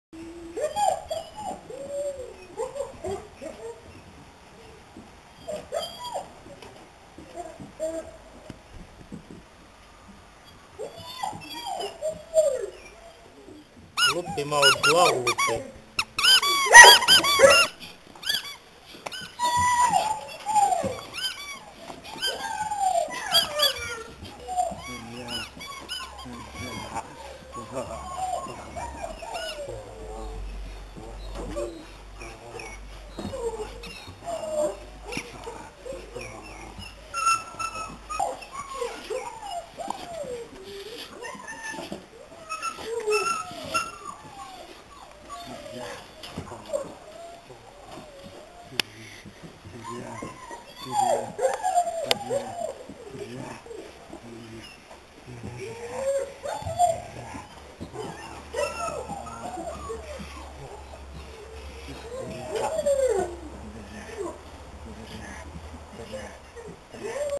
Ugo von Marbo Wolf - Presentation
Beautiful V2 BSZS Wallaby vom Kapellenberg son!!!
FOR SALE!!!

Ugo von Marbo Wolf

Top male puppy!!!
3 month's old
New Video - Stand, play and playing with the object.